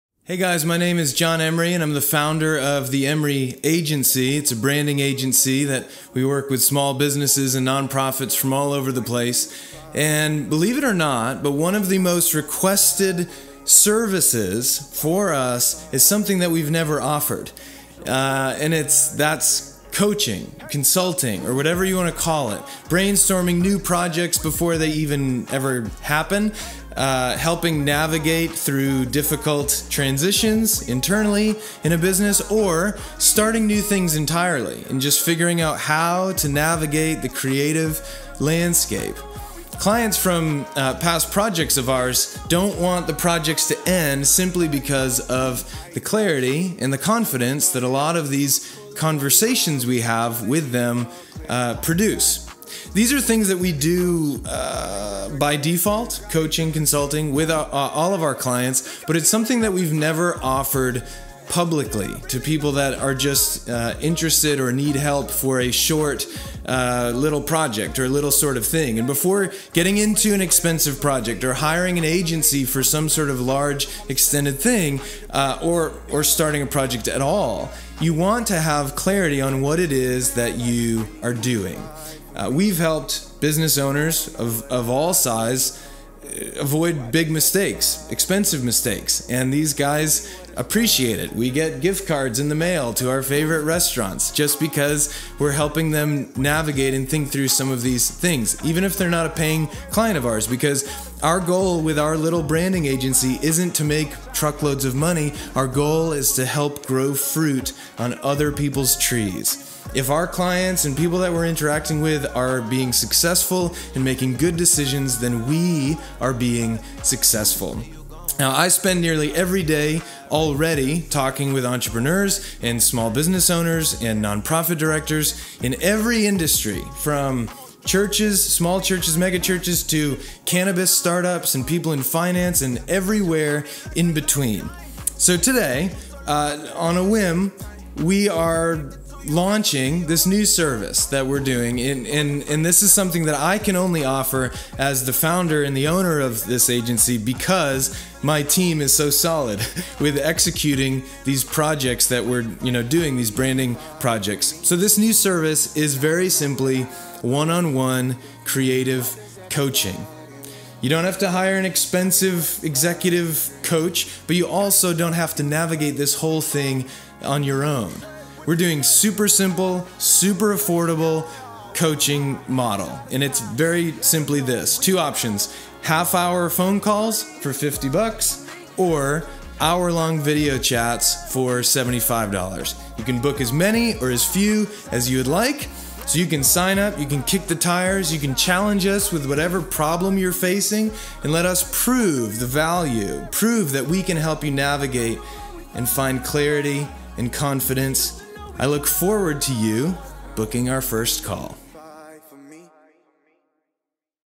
Creative Coaching with John Emery
Believe it or not, one of our most requested services is something we’ve never offered… Coaching. Consulting. Brainstorming projects before they ever happen. Helping navigate tough transitions or starting new things entirely. Clients from past projects don’t want our projects to end because of the clarity our conversations bring them.

These are things we do by default (coaching, consulting) for all of our clients, but it’s something we’ve never offered publicly. And before getting into an expensive project, or starting a project at all, you want to have clarity on what it is you’re wanting to do.

We’ve helped business owners avoid big mistakes… expensive mistakes. Even if they weren’t a paying client of ours. Because our goal isn’t to make truckloads of money, it’s to help grow fruit on other people’s trees… whatever that looks like.

I spend nearly every day talking with entrepreneurs, small business owners, non-profit directors, in every industry from the Church to cannabis startups, and everywhere in between…

Well, today marks the launch of a new service… and this is something I can only offer because my team is so solid with executing on all of our branding projects.

1-on-1 Creative Coaching.

Don’t hire an expensive executive coach, but also don’t navigate that terrain on your own.

We’re doing super-simple, super-affordable coaching models.

Two options:
* Half-hour phone calls for $50
* Hour-long video coaching for $75

You can book as many or as few as you’d like.

Sign up, kick the tires... Challenge us with whatever you’re facing and let us prove that we can help you navigate with clarity and confidence.

I look forward to you booking our first call.